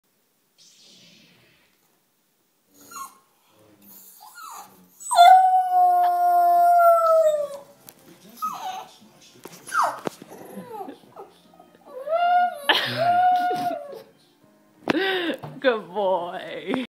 bark side response
Charlie's response to the Bark Side VW ad.